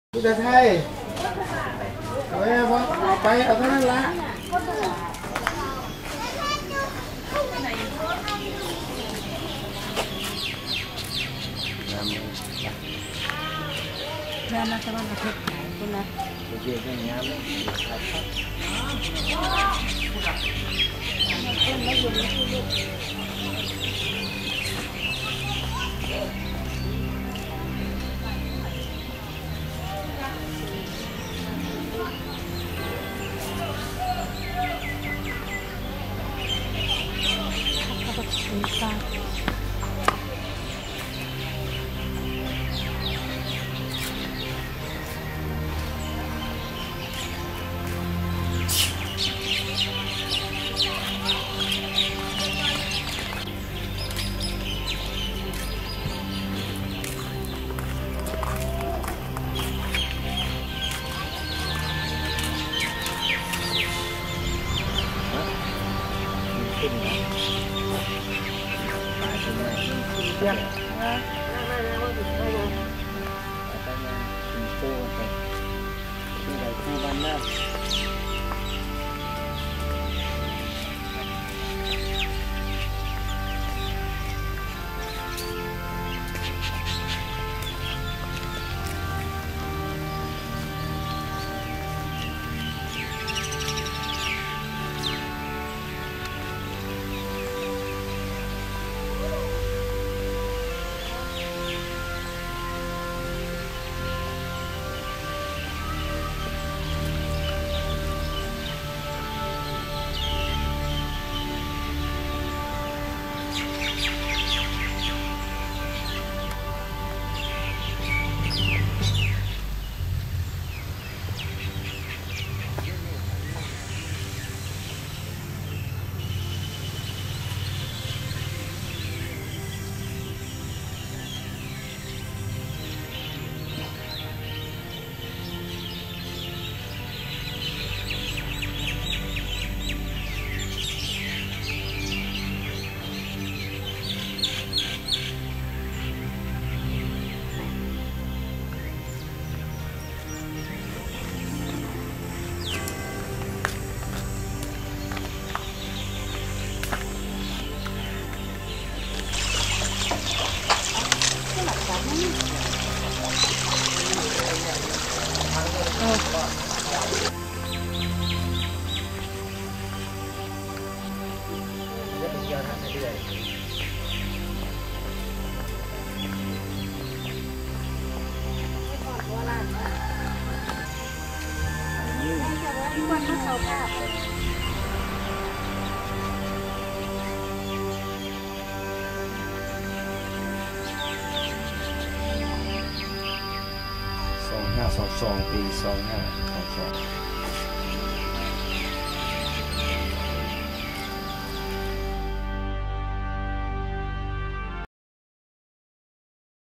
Sala KeoKu Giant Sculpture Park,Nong Khai,Thailand,30th April,2009
Sala KeoKu Sculpture Park,Nong Khai,Thailand,30th April,2009.Giant sculpture park of images inspired by Buddhism and Hinduism.Located close to Nong Khai near the Thai Lao border and The Mekong River. Fantastic giant scuptures throughout the park.Features a lake with an abundance of fish.Great tourist attraction.Costs 10 baht to enter,April 2009 หนองคายประเทศไทย